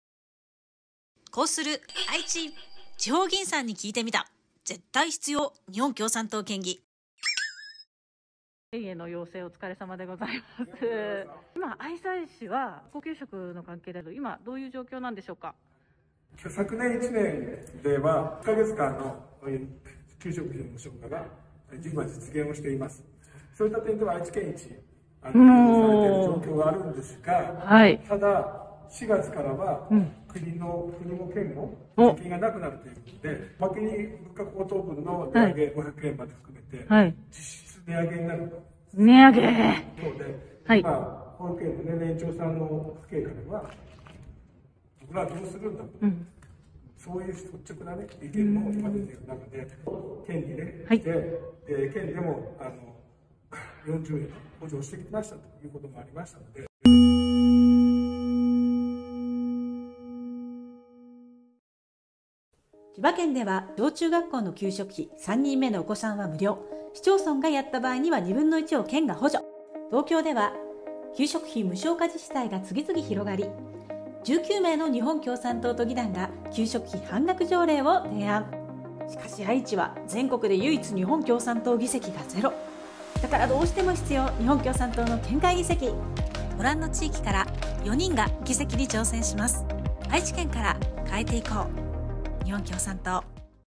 【こうする愛知】🍞学校給食問題編🍚【絶対必要日本共産党県議】
🍞学校給食問題編🍚
河合かっぺい愛西市議に聞いてみた👂

愛西市は昨年10か月間の給食無償化が実現しましたが、4月からは物価高騰分の500円まで含めて値上げに😕
保護者さんからも「これからどうするんだ」と率直な意見も出ています。
愛知県は「40円補助してきた」と言うものの、それは県立の特別支援学校と夜間高校のみで、小中学校給食費への県の補助はありません😨

千葉県では、小・中学校の給食費は3人目の子どもを無償化に。市町村が無償化した場合、2分の1を県が補助しています。また東京都では19名の日本共産党都議団が給食費半減条例を提案しています👏

愛知に 日本共産党 の県議を復活させて、学校給食無償化にしっかり取り組む県政へ💪